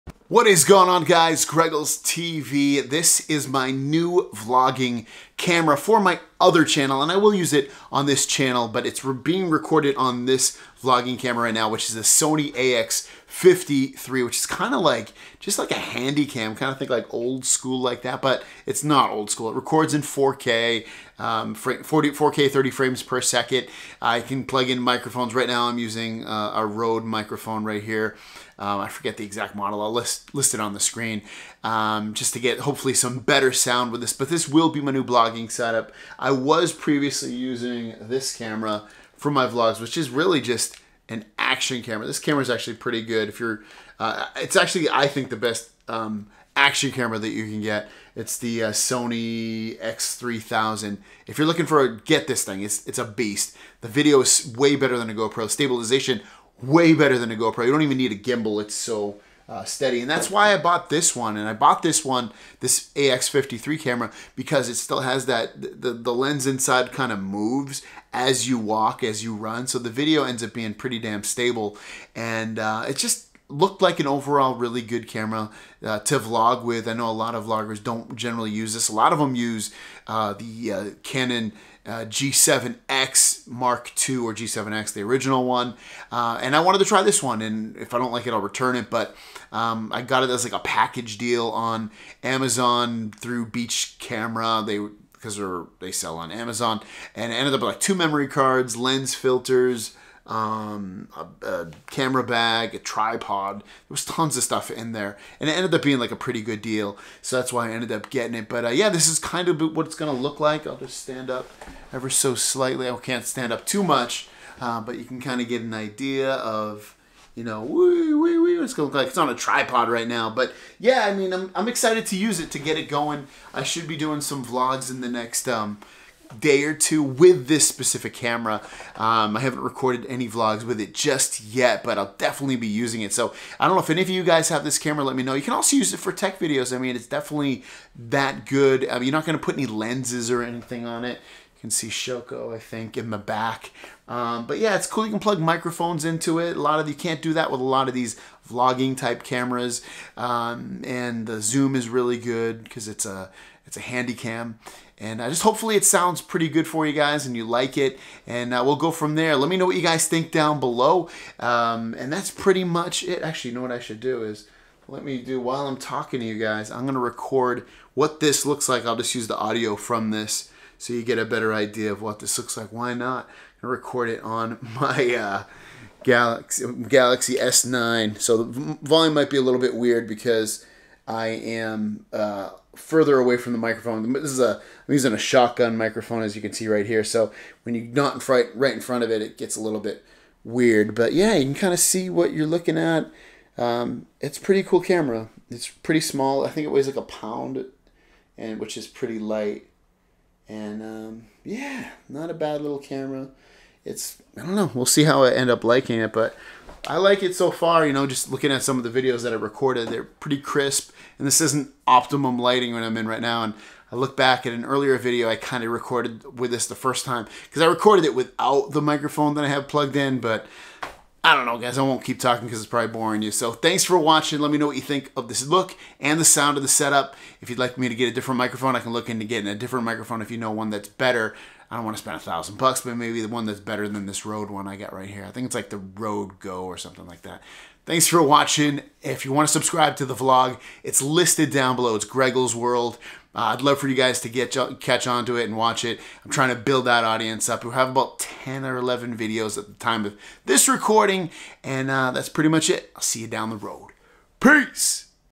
Sony AX53 MY NEW 4K VLOGGING CAMERA
I purchased a new vlogging camera, the Sony AX53 to improve the quality of my vlogging videos.  What do you guys think?

This is the Sony AX53 Bundle I purchased